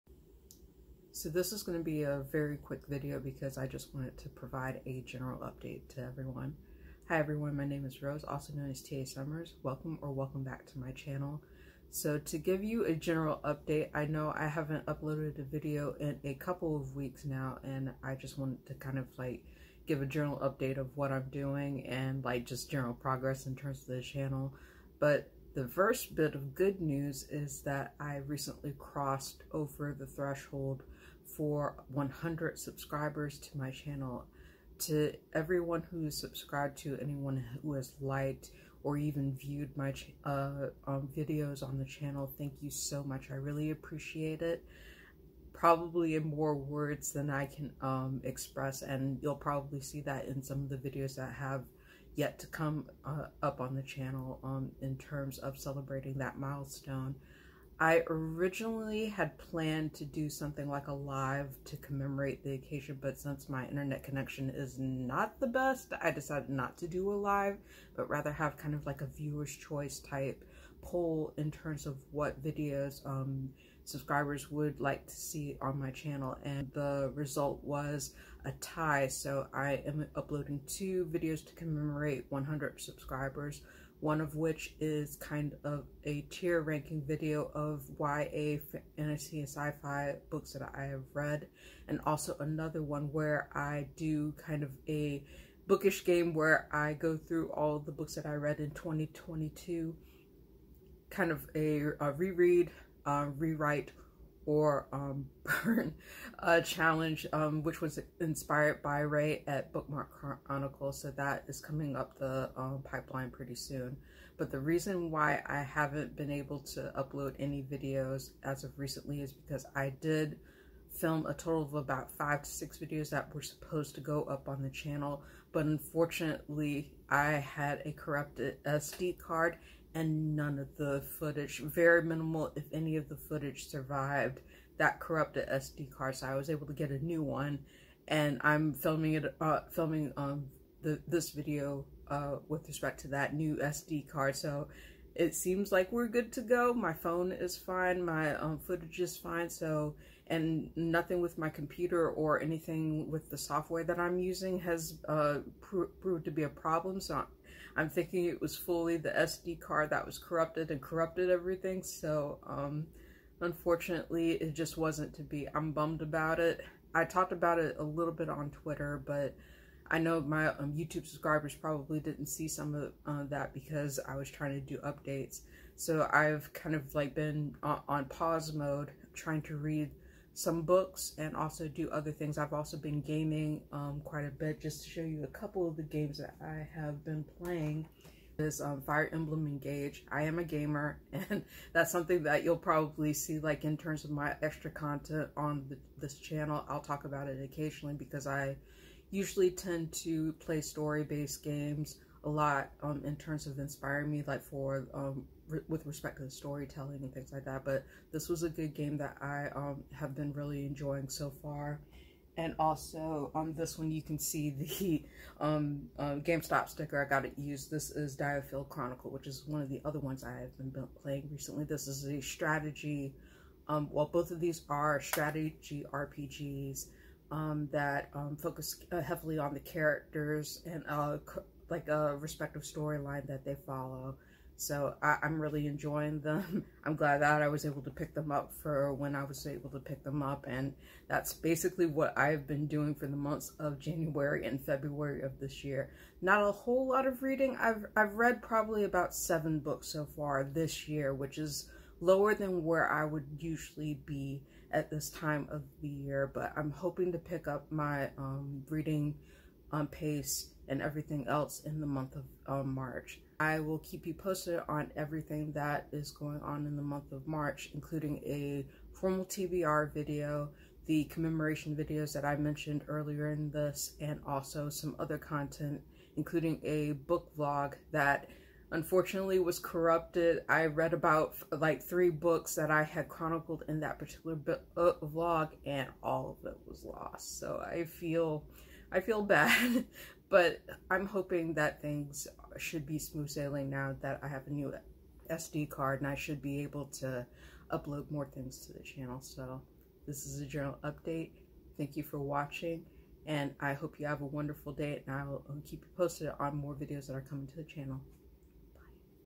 General Update - Marking 100 Subscribers, Recent Updates, and Games I'm Playing
Hi everyone, General Update video for all of you that's very short.  This is a brief explanation of why I haven't had any recent updates to the channel for videos (bad SD card = corrupted video :( ) and some other things I talk about with respect to what's coming up for videos and what I've been doing.